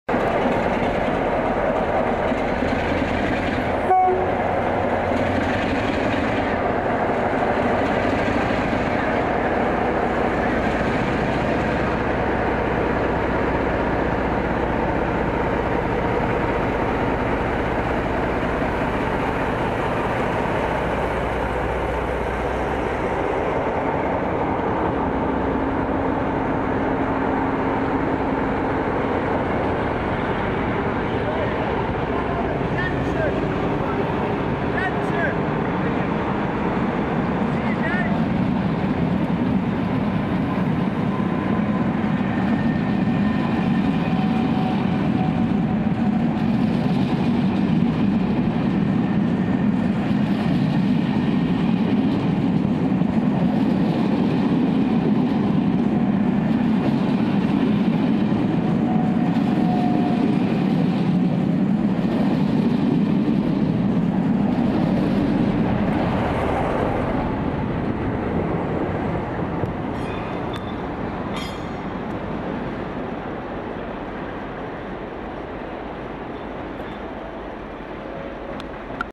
47826 1Z72 Glasgow Central - Stoke-on-Trent Compass Tours Charter 20.7.12.MOV
47826 with the old "Scarborough Spa Express" banner departs Stockport as 1Z72 Glasgow Central to Stoke on Trent returning "Compass Tours" Charter, 47854 was on the tail end of the coaching stock.